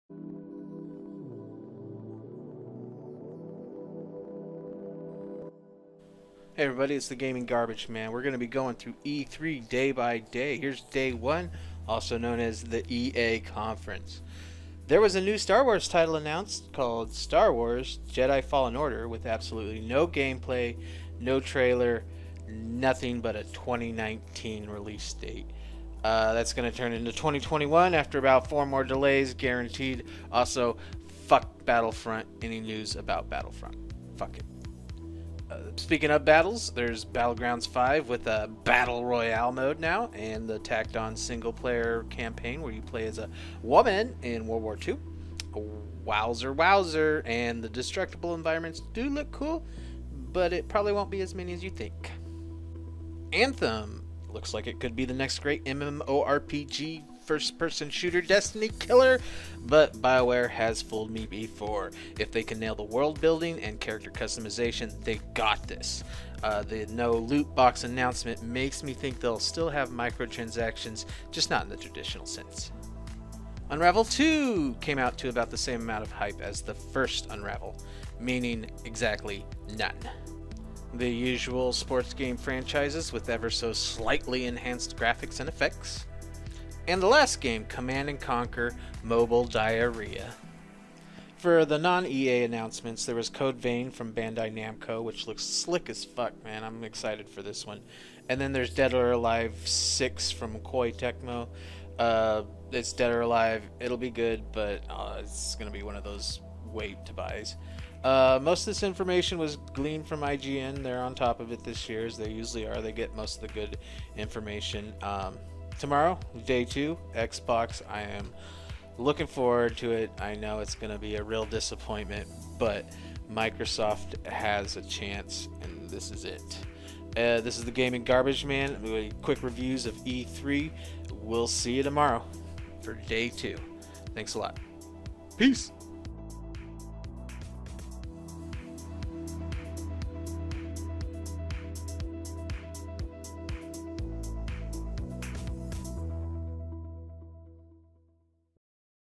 E3 2018 Day 1: EA and Code Vein
The Garbage Man got insider passes to E3!!! Join in as we go day by day, starting with June 9th's EA conference!

We look at what EA has in store and wonder about it all. Was it exciting, or just another EA show? Let's find out in this special edition Garbage Man Quickie!